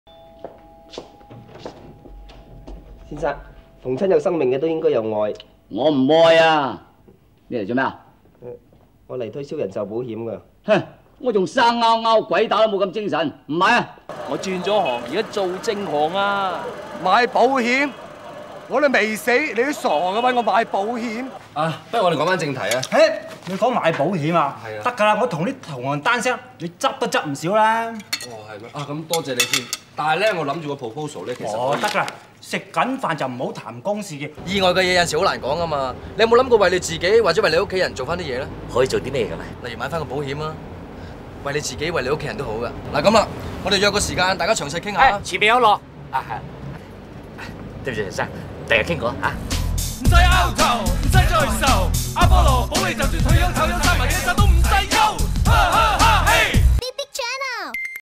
大台寶藏 | 保險 有嘻哈 | 賣保險 可以RAP到幾盡？ | 保險經紀 | 古天樂 張家輝 唐詩詠 李國麟 林嘉華 魏駿傑
【大台寶藏】保險有嘻哈 賣保險可以RAP到幾盡？！

做保險口才當然要好啦！！保險經紀「靠把口搵食」架嘛😂！！
為咗保單，古天樂、張家輝、唐詩詠、李國麟、林嘉華可以去到幾盡？
係咪可以好似魏駿傑咁保險有嘻哈？😂

想睇更多即上big big channel睇足本 賣保險可以去到幾盡？︰